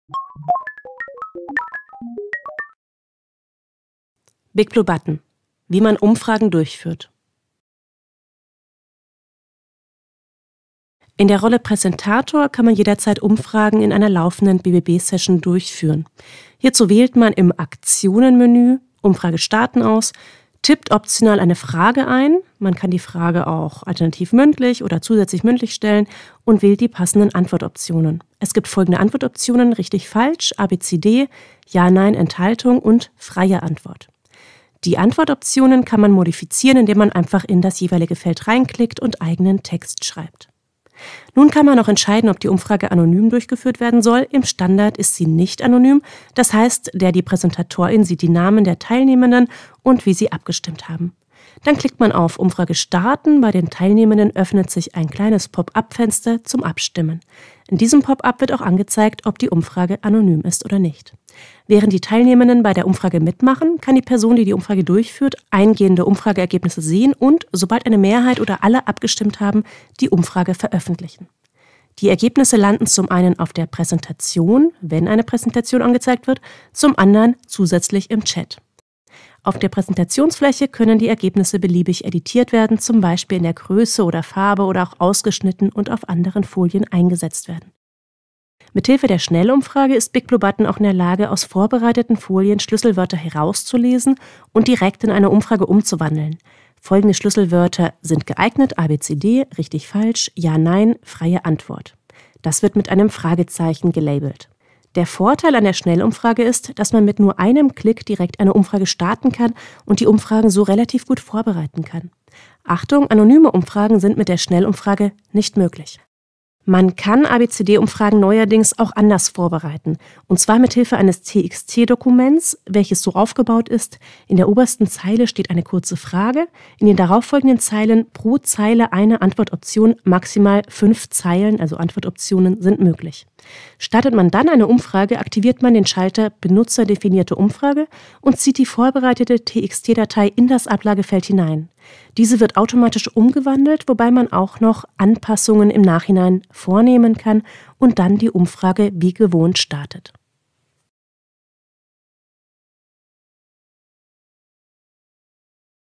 BigBlueButton | Eine Umfrage durchführen
In diesem Video wird beschrieben, wie man in der Rolle "Präsentator" in BBB eine Umfrage durchführen kann.

Kapitel:
0:00 Intro zum Durchführen von Umfragen in BBB
0:11 Wie man eine Standard-Umfrage durchführt
0:35 Antwortoptionen modifizieren
0:43 Umfragen anonym durchführen
0:56 Perspektive der Teilnehmer*innen
1:12 Präsentation der Ergebnisse
1:28 Schnellumfrage
1:56 Umfragen vorbereiten mit einer txt-Datei
2:31 Abspann

Autor: Zentrum Digitale Lehre (ZDL)
Version: BBB 2.6